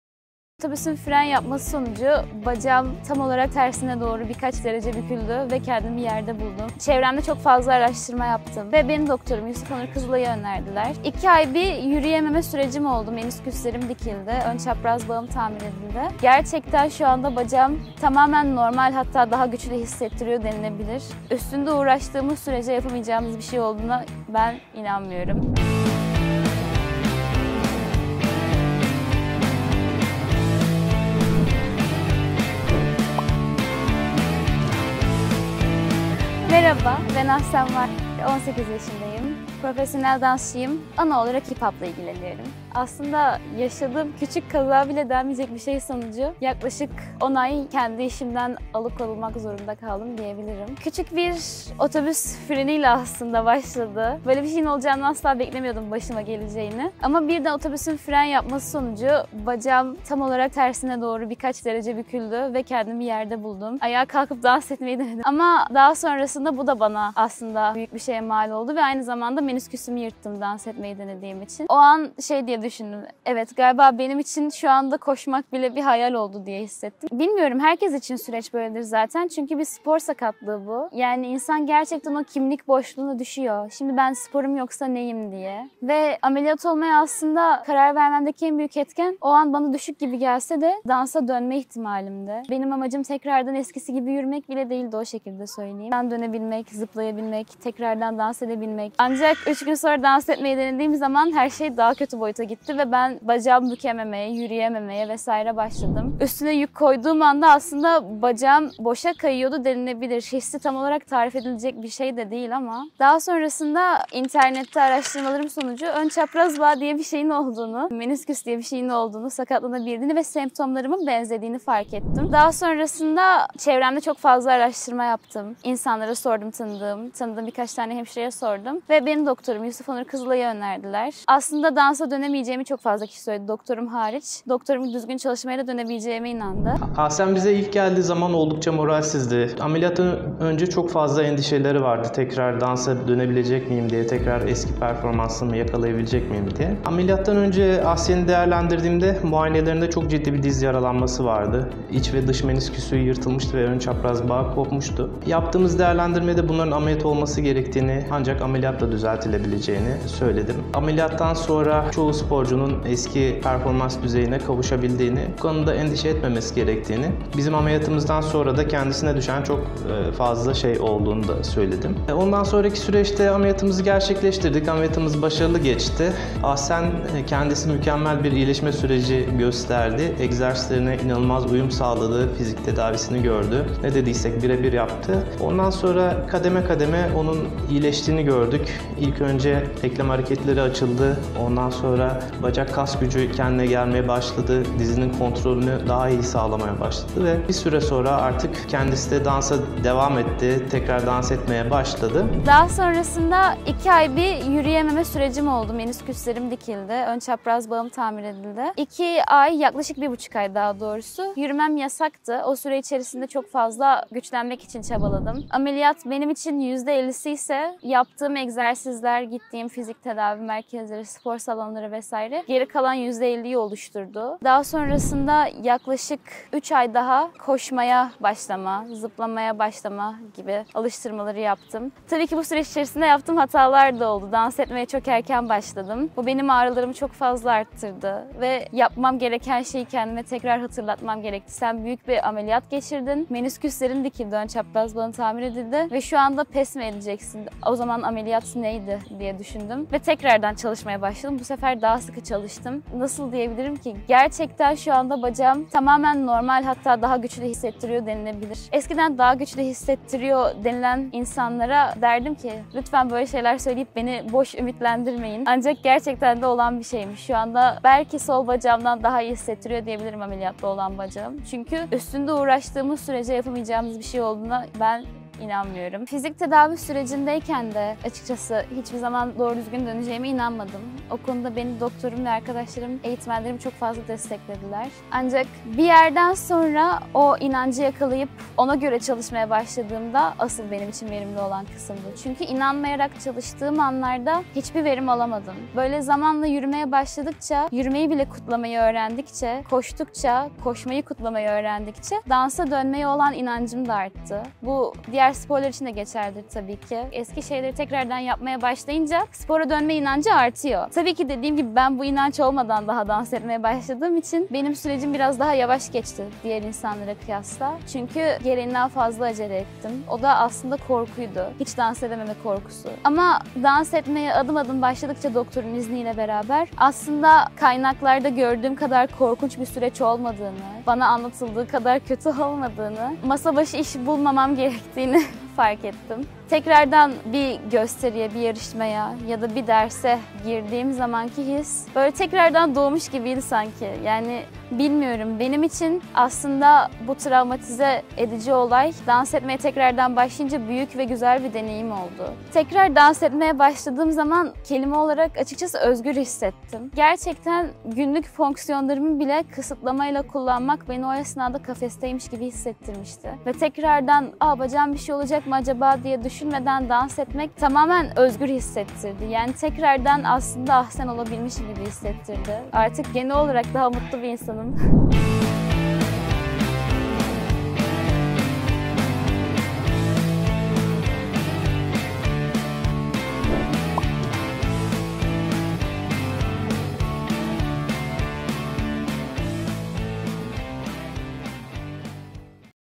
Anterior Cruciate Ligament (ACL) Reconstruction and Meniscus Repair Surgery in Turkey
She returned to dance after anterior cruciate ligament (ACL) tear & meniscus repair surgery.

The knee joint is the largest joint of the body that carries the weight of the body and enables many functions in daily life such as walking, squatting, climbing, and descending stairs. The meniscus, ligaments, and cartilage tissue in the knee joint structure collaborate together in perfect harmony.

Among the causes of knee pain, injuries, mechanical problems, various types of arthritis, and similar problems can be listed.

Knee injuries can affect the ligaments, tendons, or fluid-filled sacs that surround the knee joint, and the bone, cartilage, or ligaments that make up the joint.

Anterior cruciate ligament tears are one of the common problems almost more than half of the knee injuries. The anterior cruciate ligament is one of the 4 main ligaments in the knee that controls the forward movement of the knee.

There are symptoms such as hearing a popping or breaking sound in the knee (at the time of injury), swelling around the knee (immediately or after a few hours), difficulty and pain in putting weight on the knee, restriction in knee movements, feeling of space and insecurity in the knee.

In the knee joint, two C-shaped cartilaginous structures, inside and outside, between the femur and tibia are called the meniscus. There are symptoms such as the sudden onset of pain, swelling and tenderness in the knee, a feeling of being stuck, a feeling of locking, difficulty in bending and opening the knee, difficulty in walking, squatting, and going up and downstairs.

This is how the process of Ahsen, a professional dancer, began. Due to the sudden braking and falling of the bus, she was on, her anterior cruciate ligament was torn and her meniscus was torn. You can watch Ahsen's treatment process and recovery story in our video.